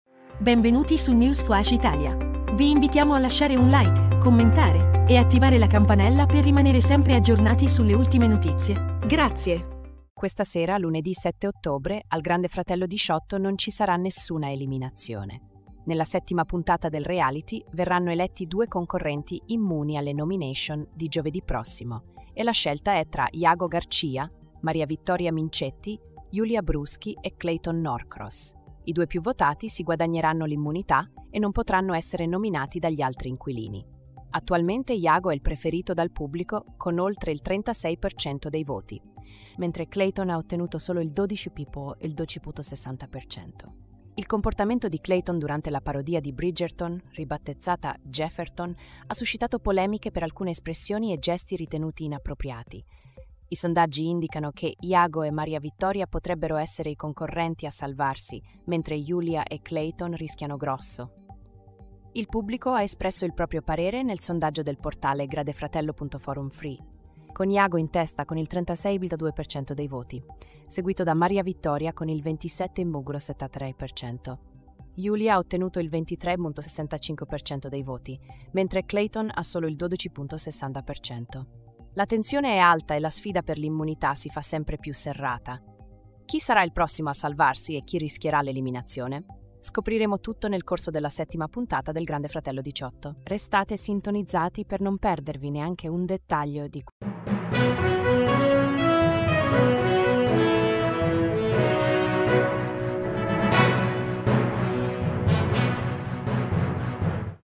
SCANDALO AL GRANDE FRATELLO 18: NOMINATI IMMUNI, MA UNO RISCHIA L'ELIMINAZIONE
Questa sera, lunedì 7 ottobre, al Grande Fratello 18 non ci sarà nessuna eliminazione. Nella settima puntata del reality verranno eletti due concorrenti immuni alle nomination di giovedì prossimo, e la scelta è tra Iago Garcia, Mariavittoria Minchetti, Yulia Bruschi e Clayton Norcross. I due più votati si guadagneranno l'immunità e non potranno essere nominati dagli altri inquilini.

Attualmente, Iago è il preferito dal pubblico con oltre il 36% dei voti, mentre Clayton ha ottenuto solo il 12.60%. Il comportamento di Clayton durante la parodia di Bridgerton, ribattezzata Giefferton, ha suscitato polemiche per alcune espressioni e gesti ritenuti inappropriati.

I sondaggi indicano che Iago e Mariavittoria potrebbero essere i concorrenti a salvarsi, mentre Yulia e Clayton rischiano grosso. Il pubblico ha espresso il proprio parere nel sondaggio del portale gradefratello.forumfree, con Iago in testa con il 36.02% dei voti, seguito da Mariavittoria con il 27.73%. Yulia ha ottenuto il 23.65% dei voti, mentre Clayton ha solo il 12.60%.

La tensione è alta e la sfida per l'immunità si fa sempre più serrata. Chi sarà il prossimo a salvarsi e chi rischierà l'eliminazione? Scopriremo tutto nel corso della settima puntata del Grande Fratello 18. Restate sintonizzati per non perdervi neanche un dettaglio di questo avvincente reality show.